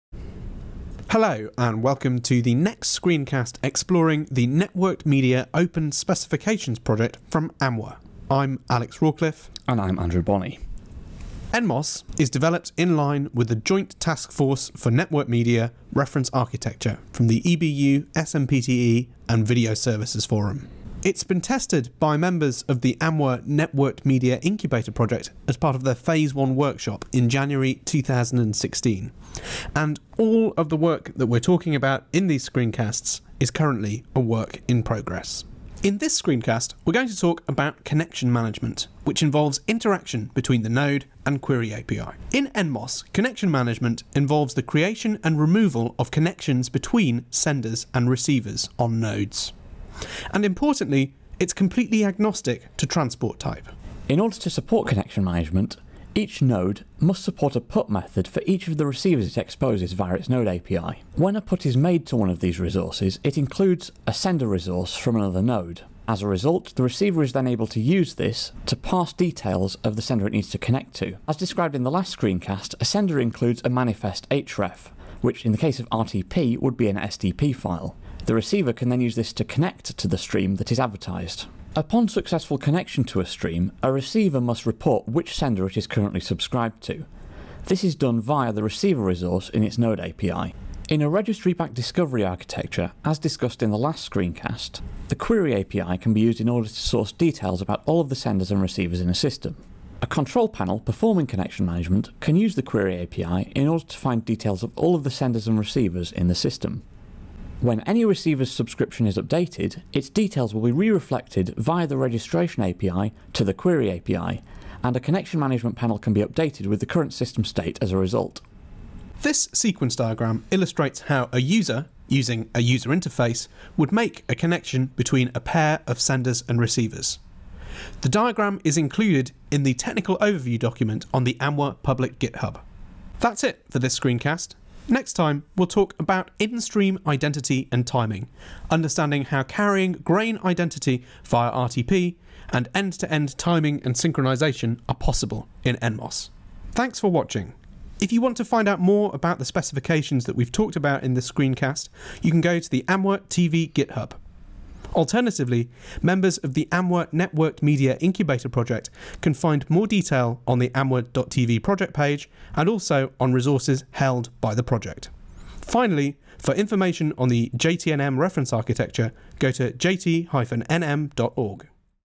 3. NMOS: Connection Mangement
NMOS allows Senders and Receivers to be connected in a transport-agnostic way.   Andrew and Alex describe how to do this using the Node API.